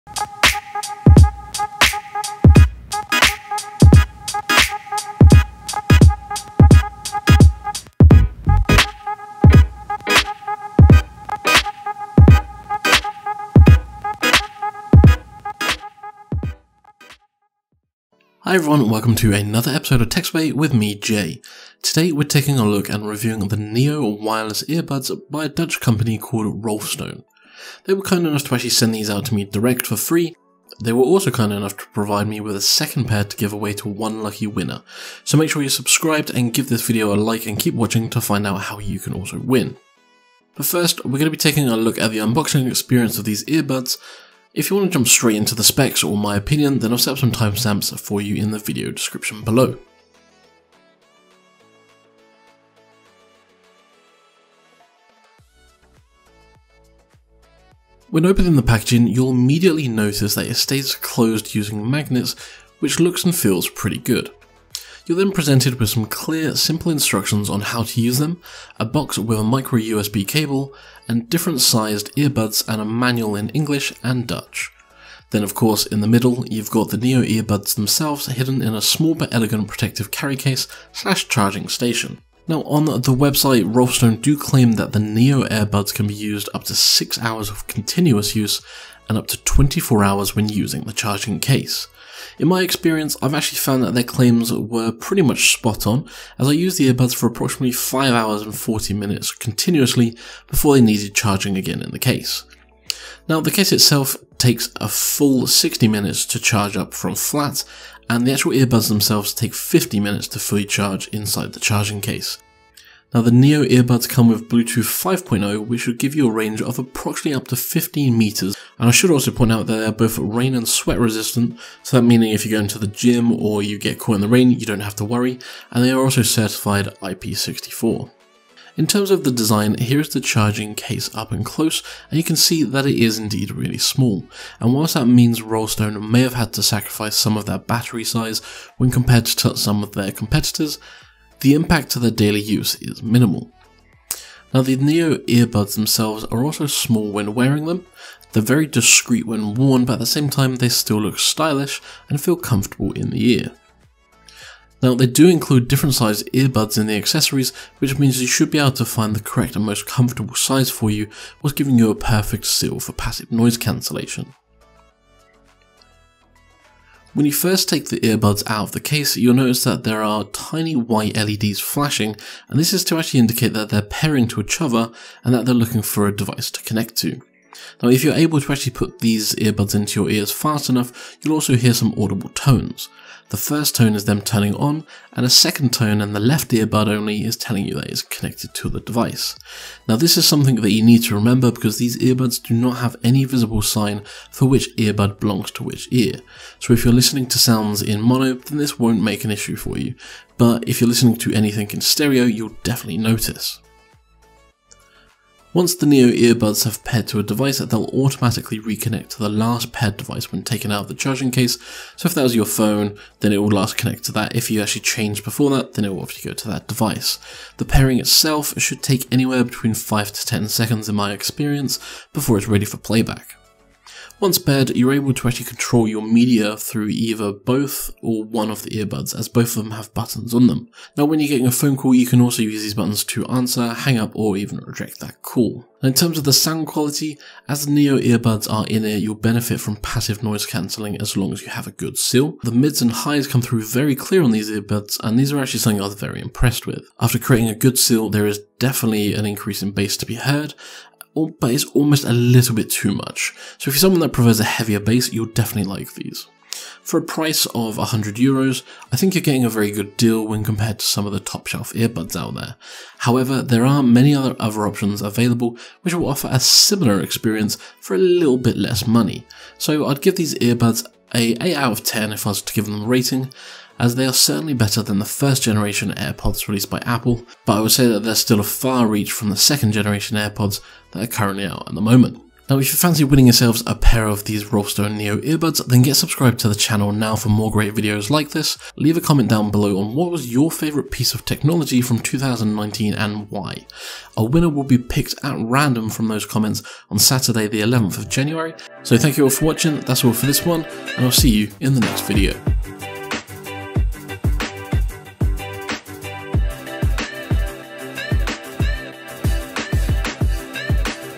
Rolfstone Neo - First impression / Review - Fully wireless earbuds
Rolfstone Neo Truly Wireless Earbuds - In this video I go over my first impressions, product review and a fantastic giveaway for subscribers.

These Rolfstone Neo headphones were sent to me free of charge in exhange for a honest review. Tech Sway will always provide its honest feedback on produts whether positive or negative.